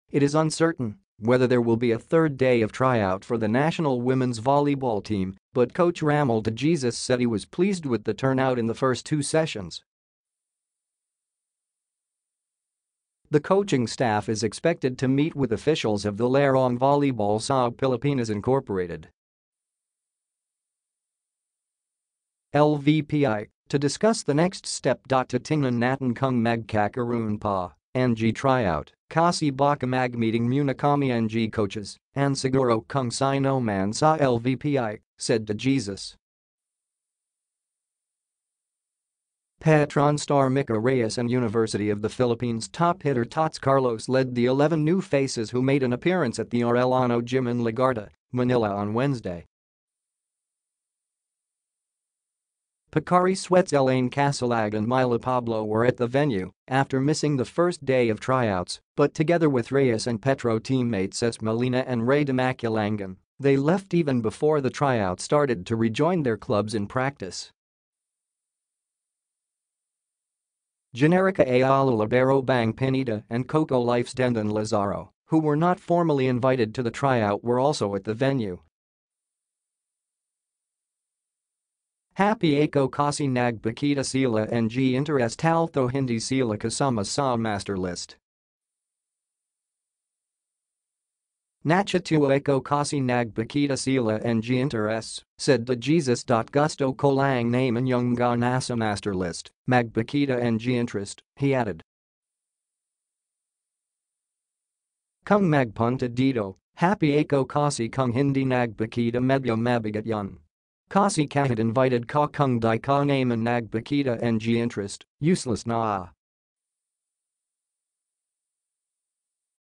Ramil de Jesus pleased with interest shown by top volleyball stars in PH team tryouts
IT is uncertain whether there will be a third day of tryout for the national women’s volleyball team, but coach Ramil de Jesus said he was pleased with the turnout in the first two sessions.

The coaching staff is expected to meet with officials of the Larong Volleyball sa Pilipinas Inc. (LVPI) to discuss the next step.

“Titingnan natin kung magkakaroon pa (ng tryout) kasi baka mag-meeting muna kami ng coaches and siguro kung sino man sa LVPI,” said De Jesus.

Petron star Mika Reyes and University of the Philippines top hitter Tots Carlos led the 11 new faces who made an appearance at the Arellano gym in Legarda, Manila on Wednesday.

Pocari Sweat’s Ellaine Kasilag and Myla Pablo were at the venue after missing the first day of tryouts, but together with Reyes and Petro teammates Ces Molina and Rhea Dimaculangan, they left even before the tryout started to rejoin their clubs in practice.

Generika-Ayala libero Bang Pineda and Cocolife’s Denden Lazaro, who were not formally invited to the tryout , were also at the venue.

“Happy ako kasi nagpakita sila ng interes talthough hindi sila kasama sa master list. Natutuwa ako kasi nagpakita sila ng interes,” said de Jesus.

“Gusto ko lang naman ‘yung mga nasa master list, magpakita ng interest,” he added. “Kung magpunta dito, happy ako kasi kung hindi nagpakita medyo mabigat ‘yun. Kasi kahit invited ka kung ‘di ka naman nagpakita ng interest, useless na.”